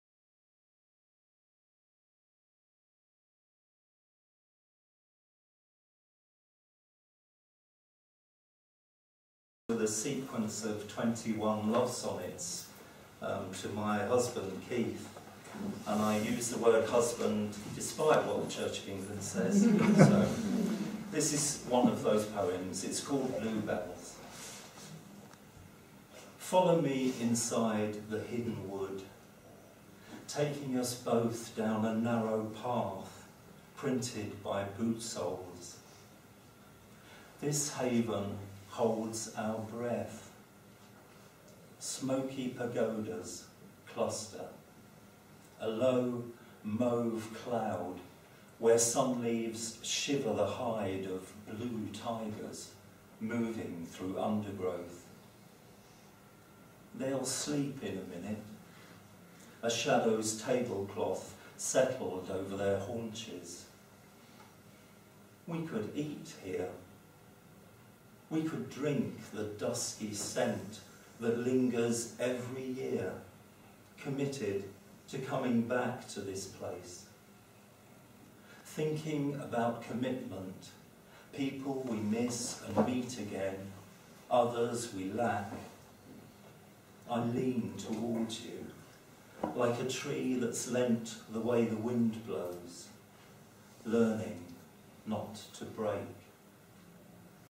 HambergerPoem 10 'Bluebells'Lewes2012
ROBERT HAMBERGER POEM
2012 Lewes - The Needlemakers

A poetry reading by Robert Hamberger

10th of 12 poems:
'Bluebells'

Poem from 'The Smug Bridegroom' by Five Leaves 2002

Part of a Needlewriters Reading